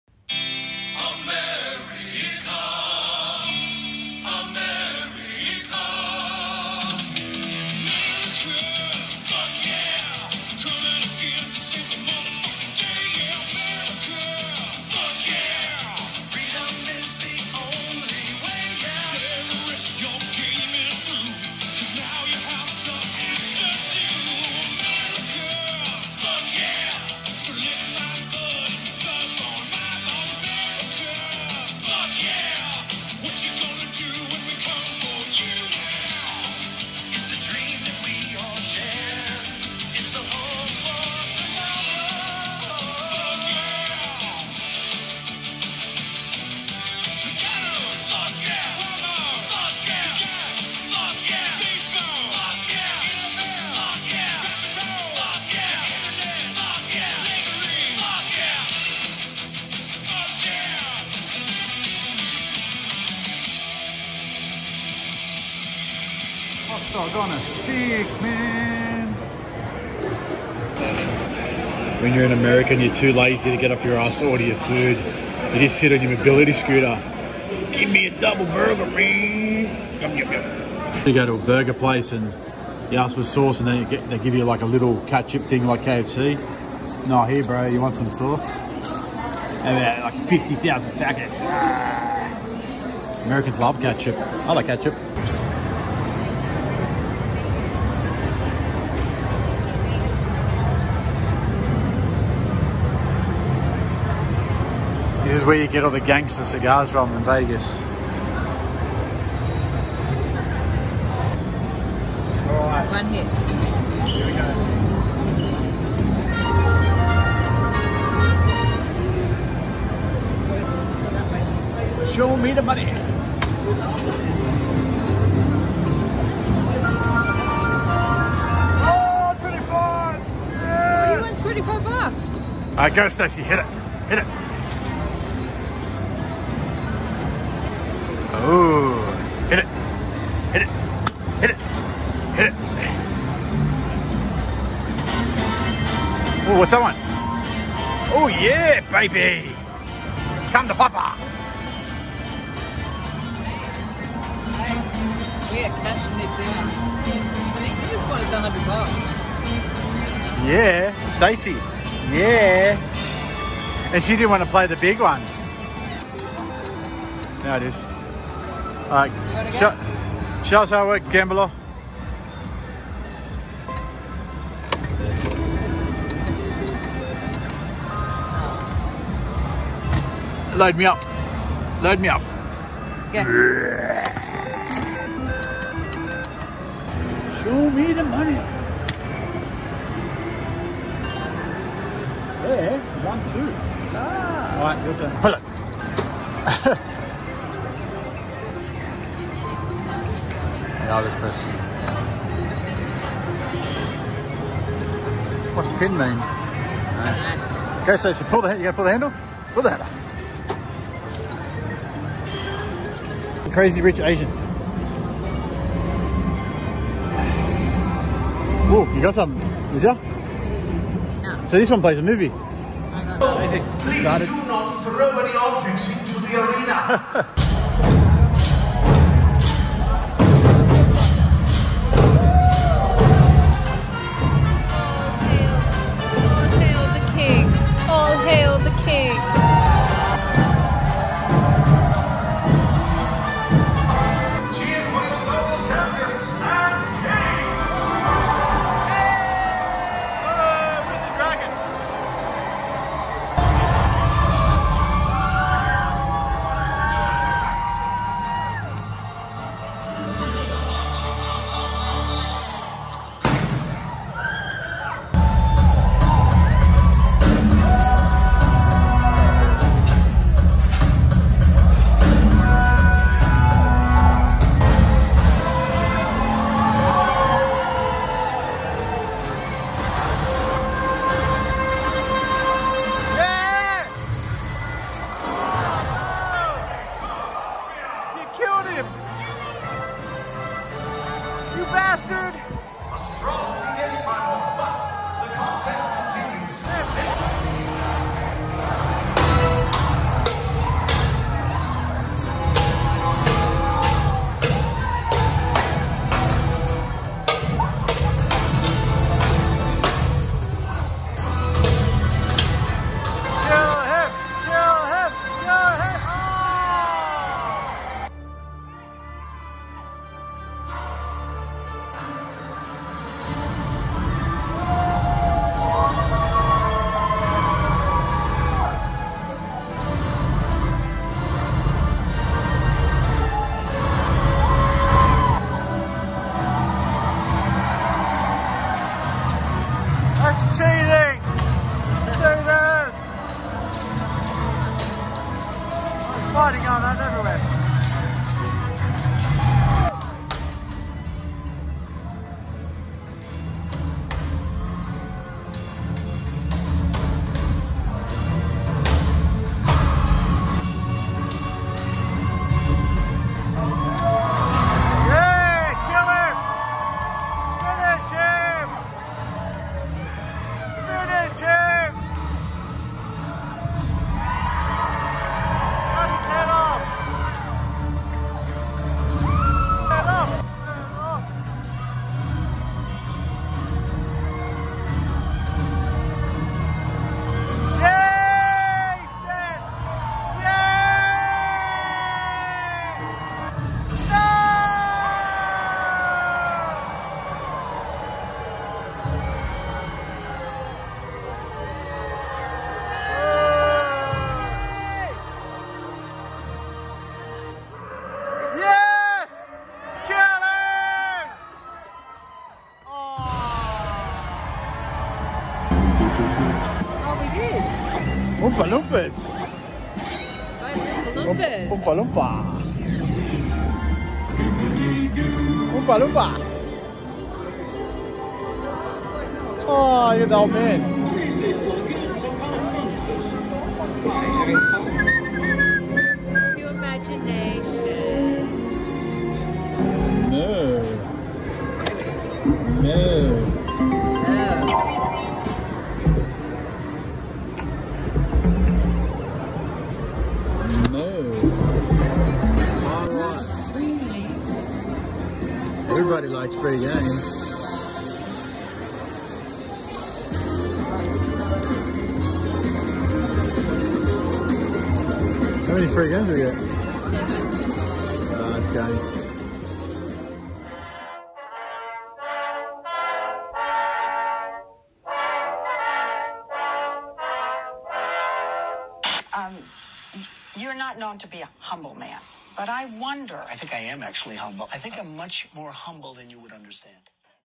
Las Vegas Holiday part 1
Video from our Las Vegas holiday broken up in parts showing our fight from Sydney to LA, then arriving at Excalibur Hotel where we play some pokies and had dinner & show watching the Knights battle it our in the Excalibur Arena - so much video take so it’s broken up in parts